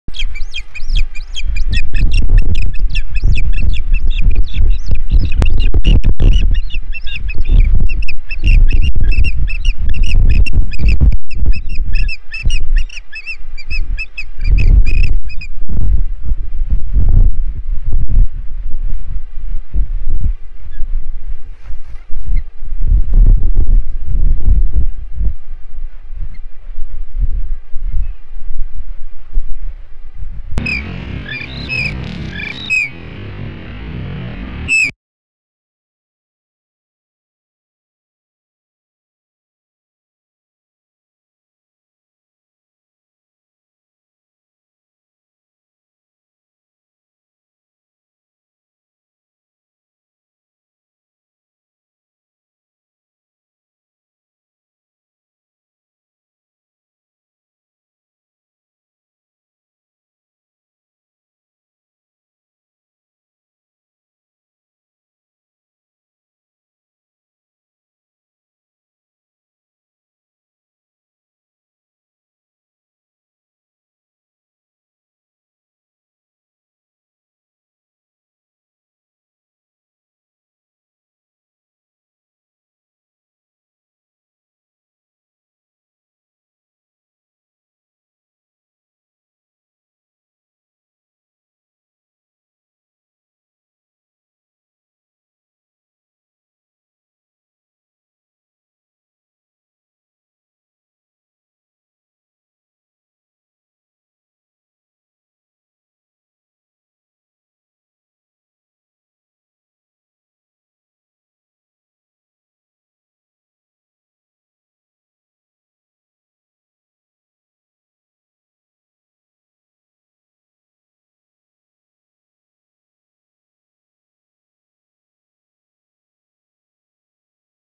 SteveWithTwoFish, BothCams, June23,2019 HogIsland Osprey Audubon
Steve delivers two fish, one in each foot/Talons. Rachel takes one fish, and the other fish ends up in Steve's beak. As he flies off, you can see him trying to grab that fish with his talons. Audubon Osprey Nest, Hog Island (cam powered by EXPLORE.org)